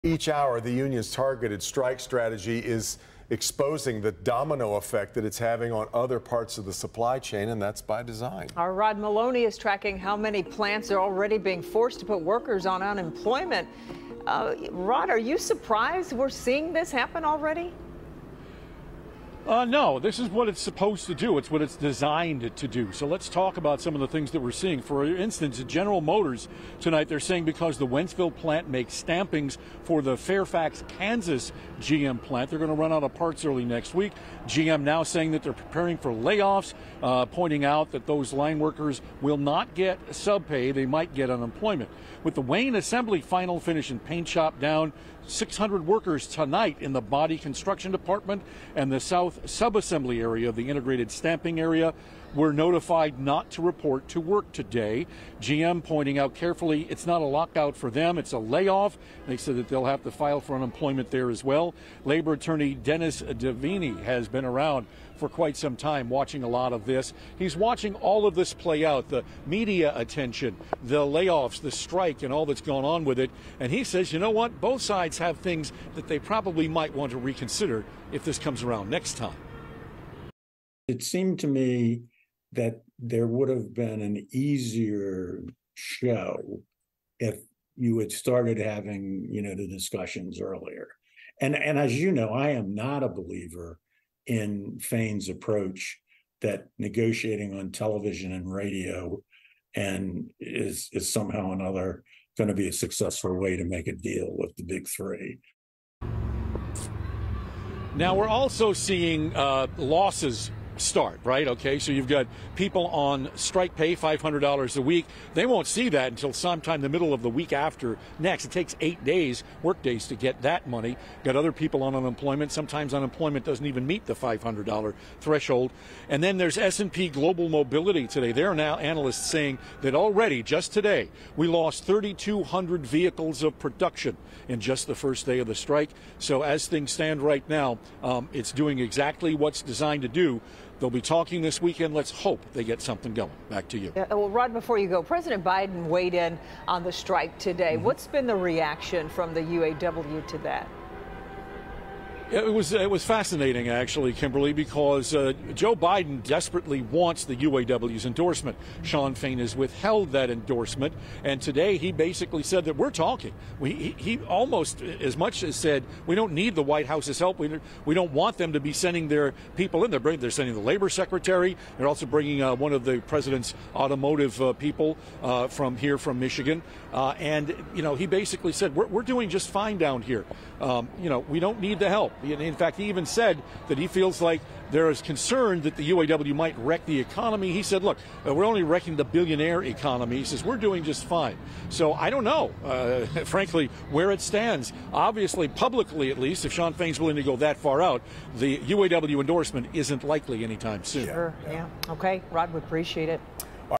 Here's where things stand on the UAW strike
The union's targeted strike strategy exposes its domino effect on other parts of the supply chain.